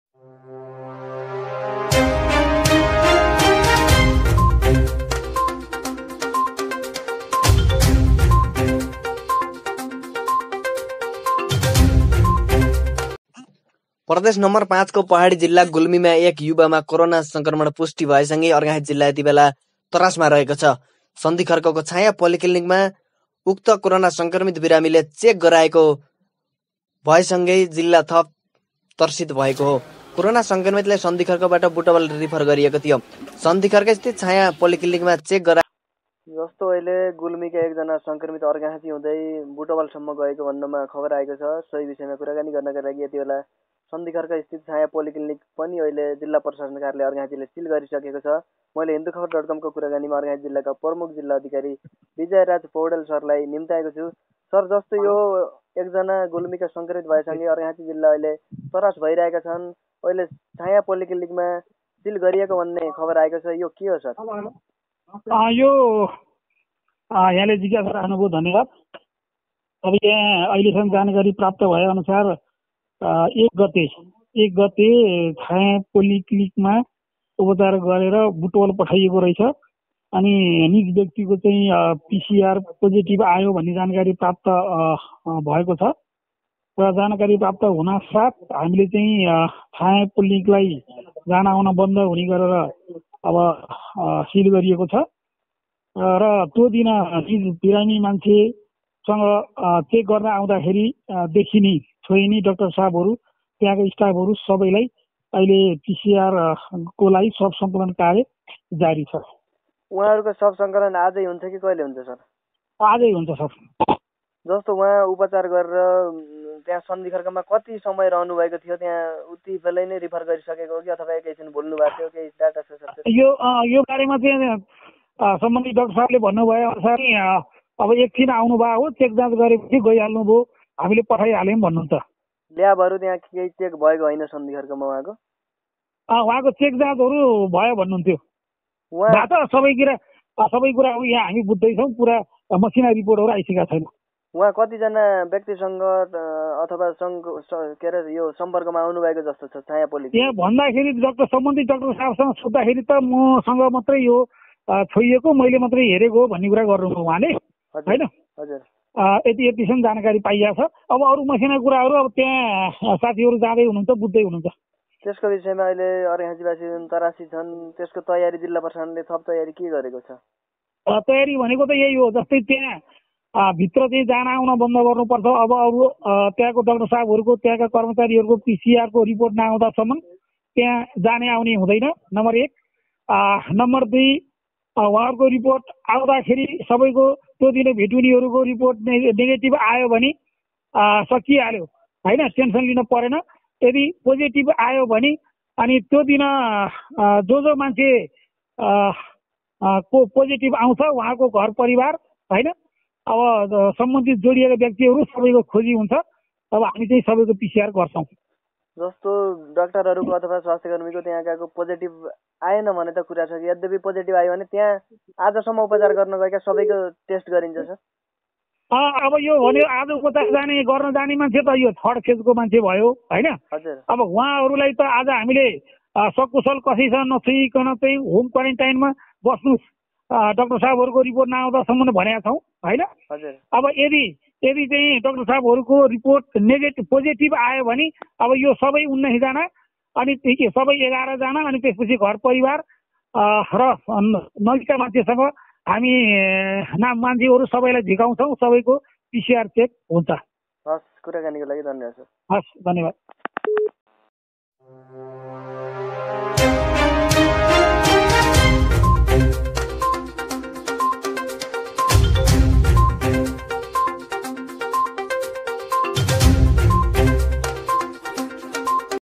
अर्घाखाँचीमा त्रास ! छायाँ पाेलिक्लनिक शिल, प्रमुख जिल्ला अधिकारीसँग टेलिफोनमा कुराकानी !
हाम्रो यस अफिसियल Webside मा गएर हरेक समाचार पढ्न सक्नुहुनेछ ।